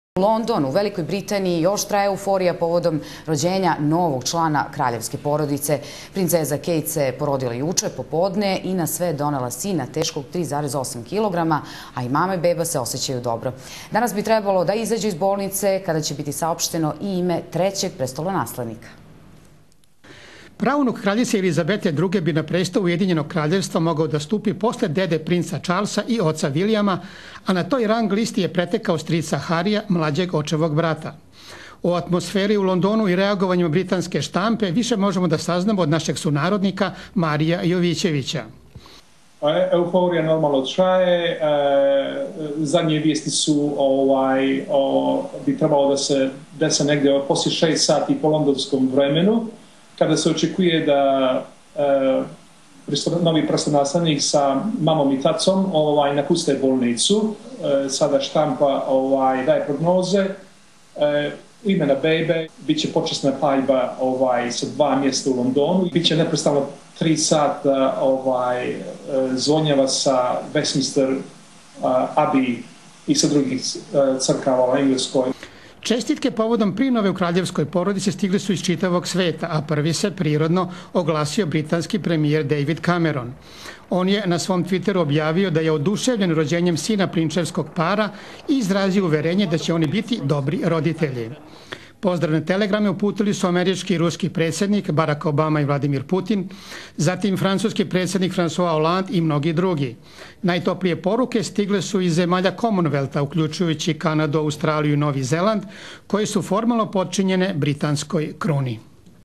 Princeza rodila sina
Juče se rodio se sin princa Vilijama i princeze Kejt! Dečak je zdrav i težak 3,8 kilograma, a rodio se u 16:24 (GMT). Princeza se oseća odlično i ostala je u bolnici preko noći.

Izvor: Vojvođanski dnevnik, RTV, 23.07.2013.